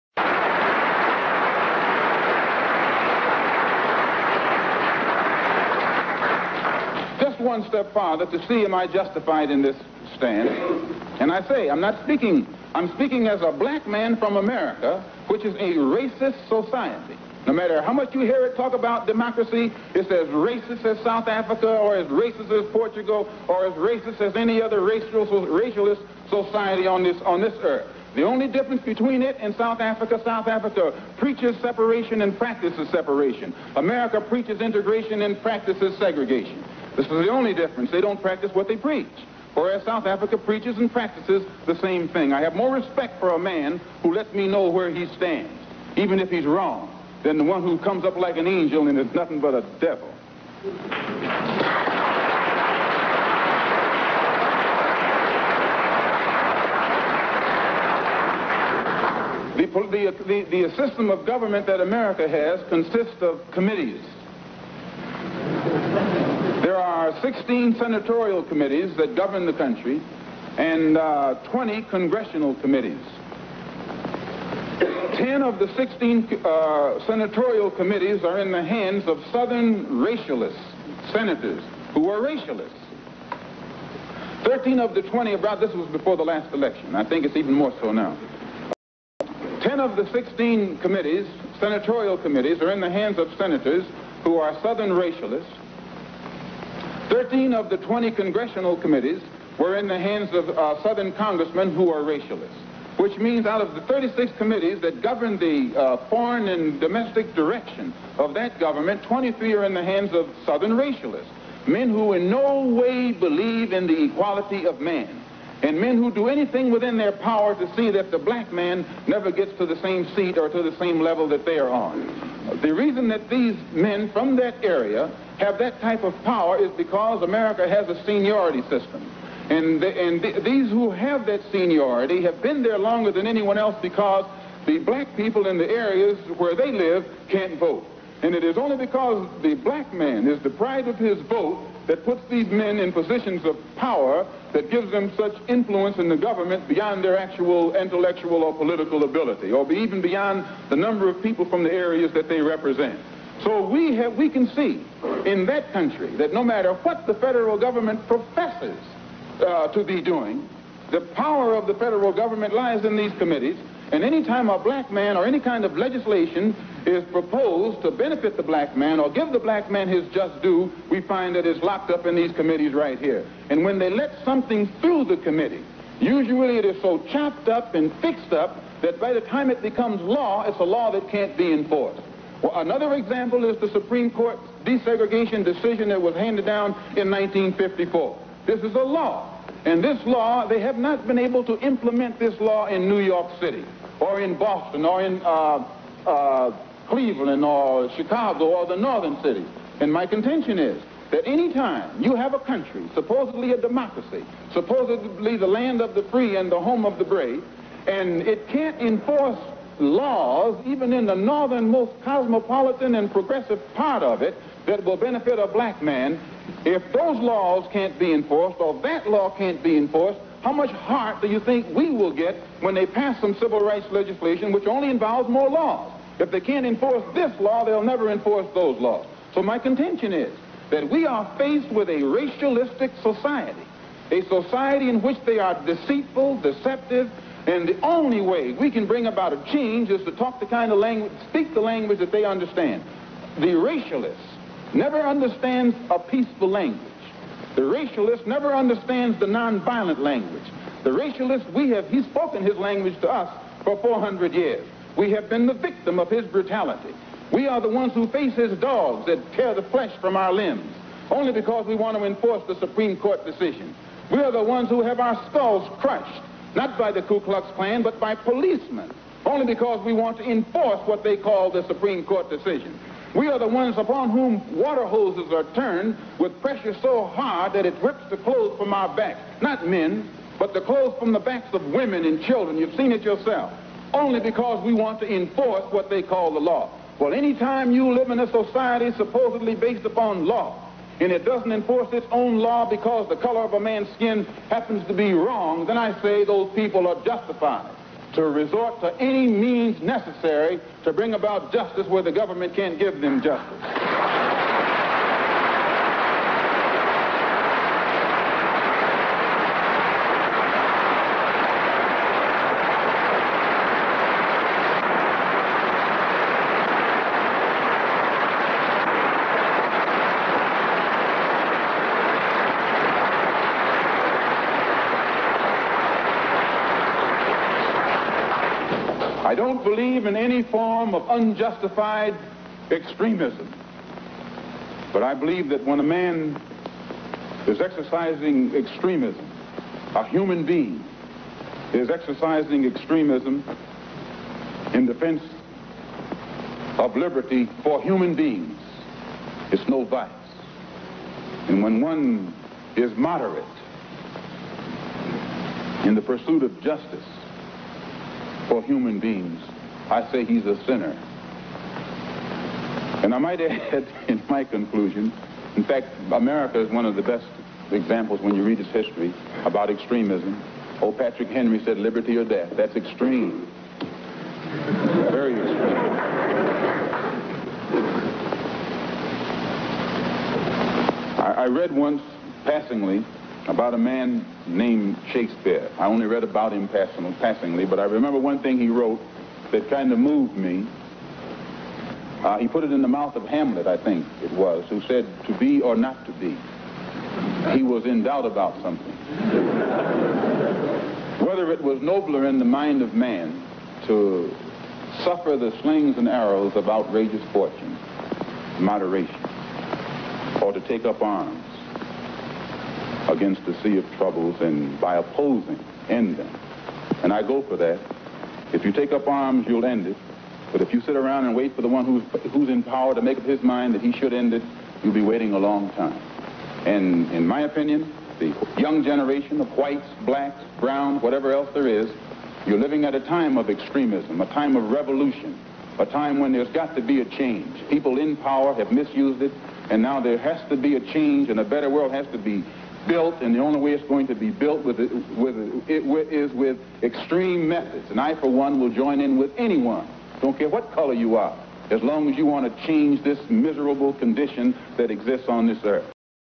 Malclom X Speech at Oxford University
Malcolm C speaks at Oxford University on Racism.